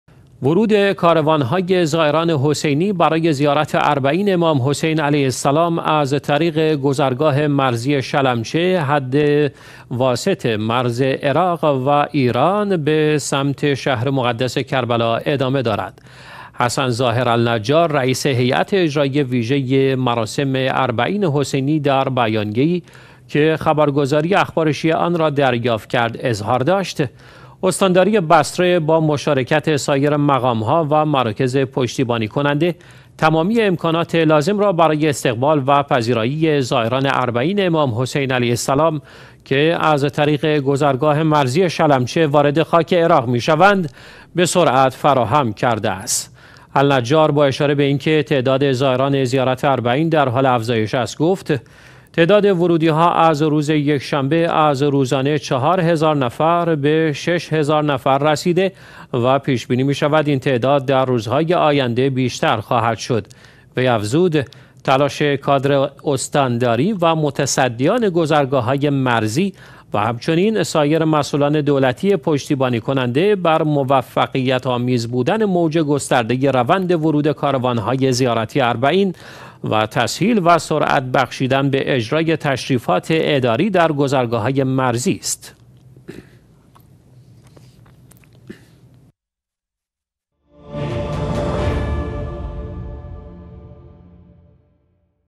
افزایش روزافزون زائران اربعین امام حسین علیه السلام از گذرگاه مرزی شلمچه
ورود کاروان‌های زائران حسینی برای زیارت اربعین امام حسین علیه‌ السلام از طریق گذرگاه مرزی شلمچه حد واسط مرز عراق و ایران به سمت شهر مقدس کربلا ادامه دارد.
حسن ظاهر النجار، رئیس هیات اجرائی ویژه مراسم اربعین حسینی در بیانیه ای که خبرگزاری اخبار شیعه آن را دریافت کرد، اظهار داشت: استانداری بصره با مشارکت سایر مقام ها و مراکز پشتیبانی کننده، تمامی امکانات لازم را برای استقبال و پذیرائی زائران اربعین امام حسین علیه السلام که از طریق گذرگاه مرزی شلمچه وارد خاک عراق می شوند، به سرعت فراهم کرده است.
النجار با اشاره به اینکه تعداد زائران زیارت اربعین در حال افزایش است، گفت: تعداد ورودی ها از روز یکشنبه، از روزانه چهار هزار نفر به شش هزار نفر رسیده و پیش بینی می شود این تعداد در روزهای آینده بیشتر خواهد شد.
وی افزود: تلاش کادر استانداری و متصدیان گذرگاه های مرزی و هم چنین سایر مسئولان دولتی پشتیبان کننده بر موفقیت‌آمیز بودن موج گسترده روند ورود کاروان های زیارتی اربعین، و تسهیل و سرعت بخشیدن به اجرای تشریفات اداری در گذرگاه های مرزی است.